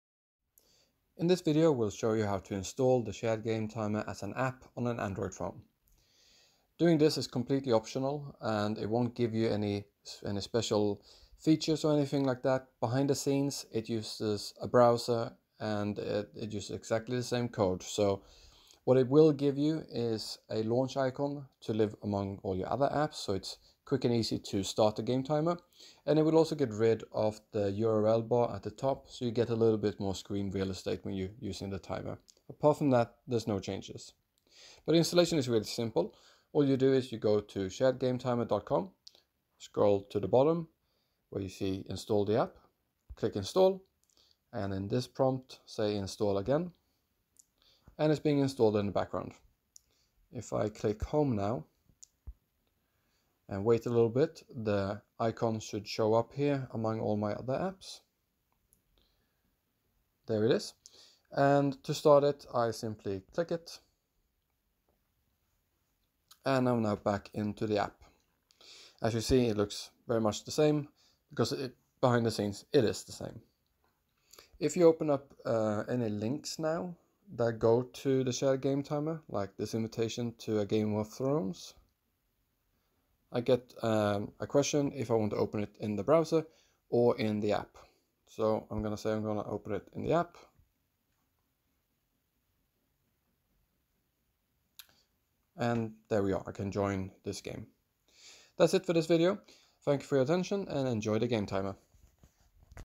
Installing the app on Android
Shows how to install the app on an Android phone.